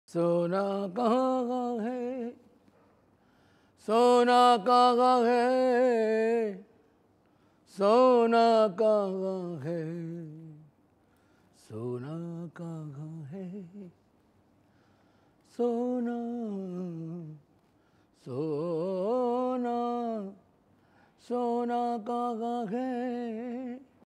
EXCLUSIVE: Bappi Da spotted without Gold
Disturbed by the disappearance of his gold, Bappi Lahiri just can’t stop thinking about it. He can’t seem to find the right lyrics to his songs either.
One question that is on everyone’s minds, Sona Kahan Hai?